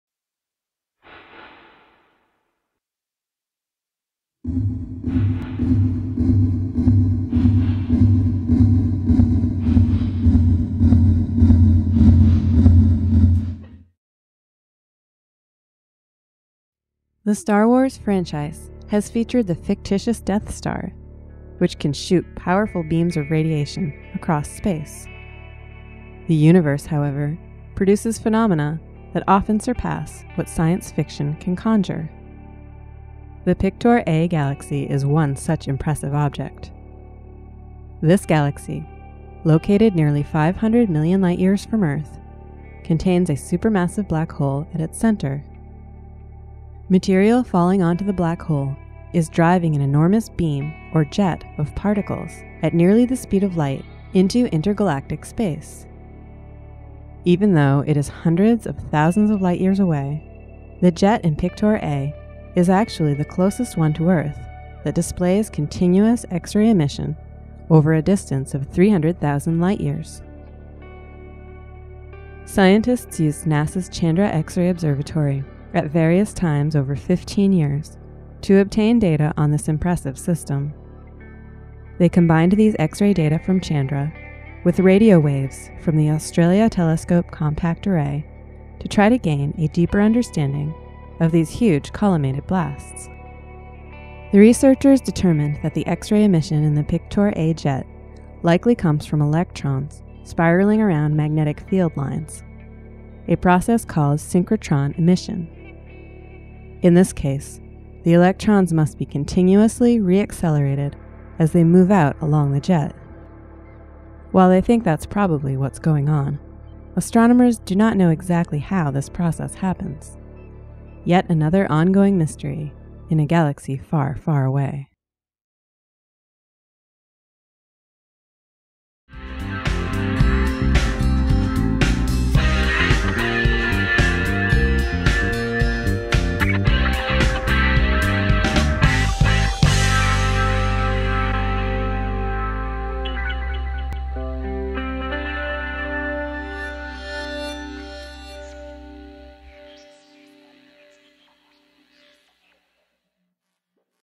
Black Hole Sends a "Death Star" Beam Into Deep Space | Video
- a supermassive black hole in the Pictor A galaxy is emitting a powerful jet of electrons covering thousands of light-years.